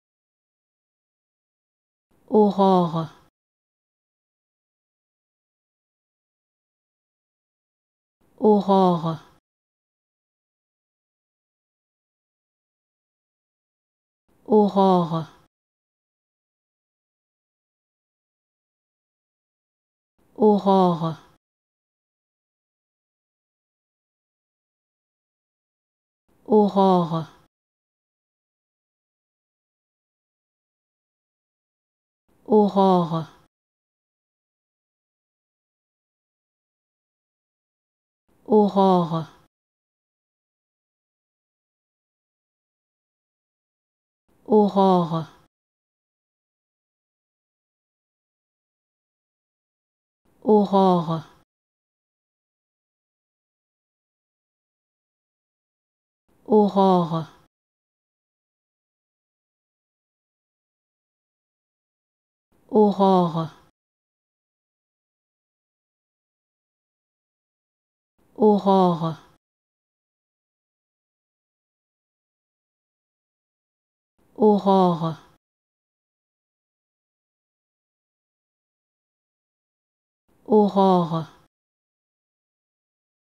How to Pronounce ''Aurore'' Correctly in French
Learn how to say and properly pronounce ''Aurore'' in French with this free pronunciation tutorial. Apprenez à prononcer des mots en français grâce à nos tutoriels.